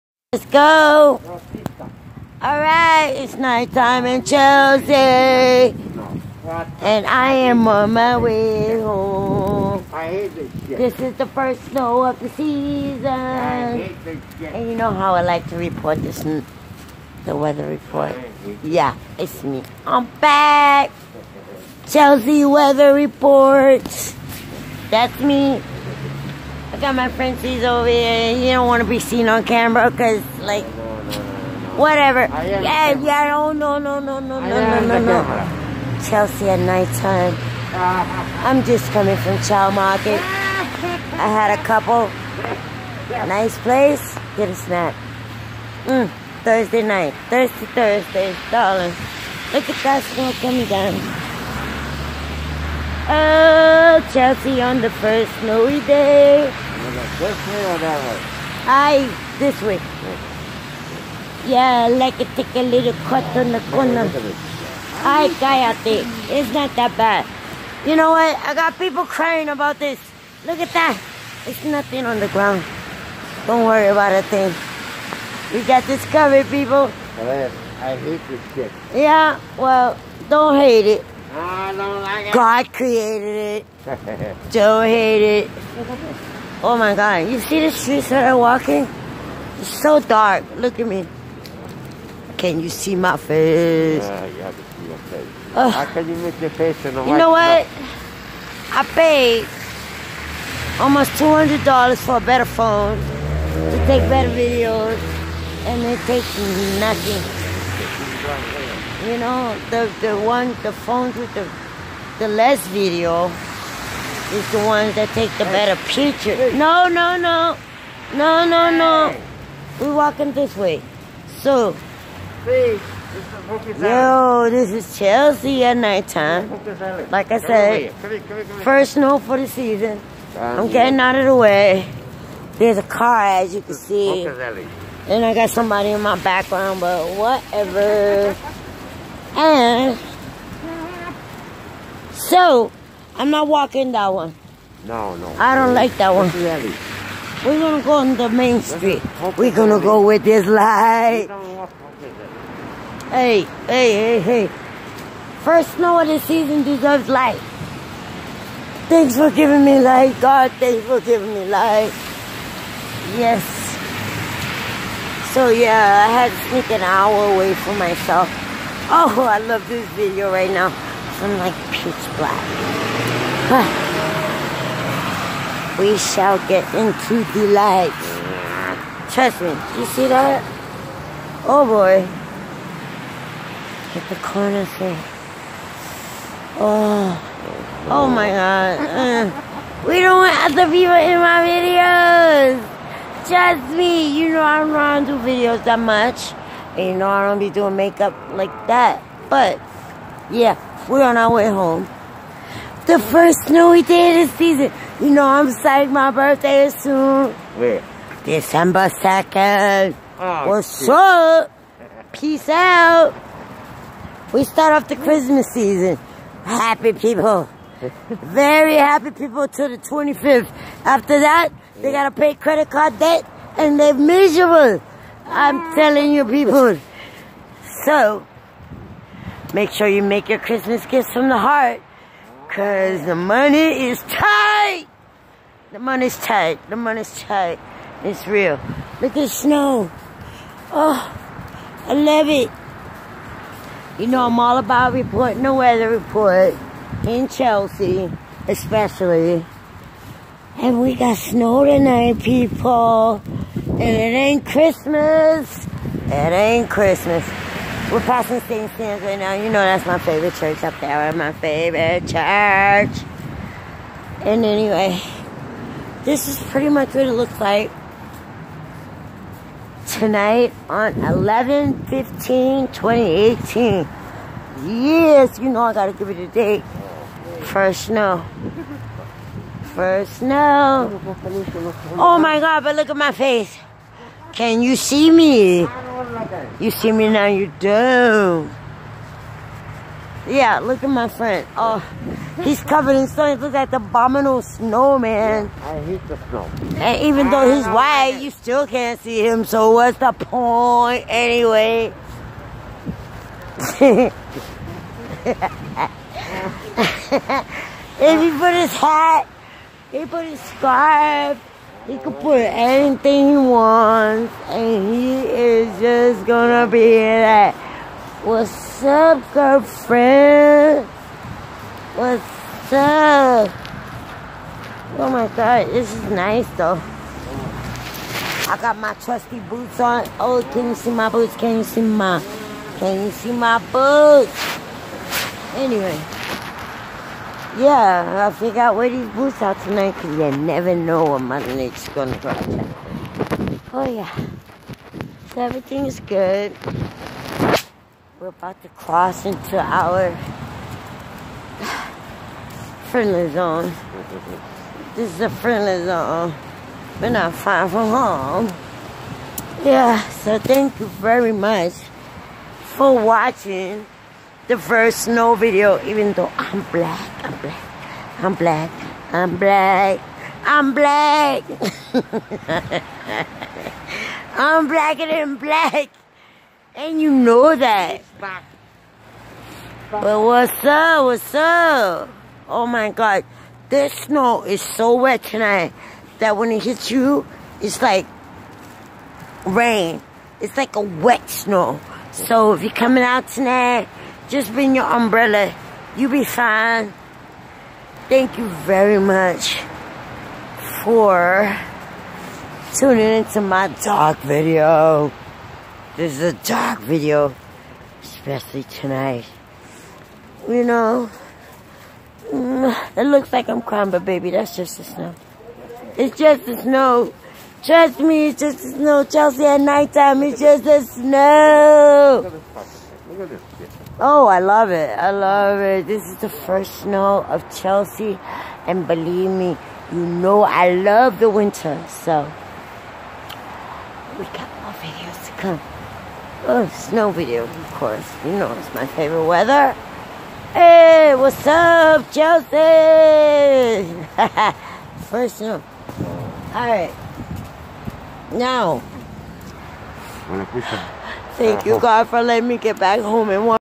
Chelsea Massachusetts first snow fall.
I will be reporting the winter weather my favorite time of year. More video to come.❤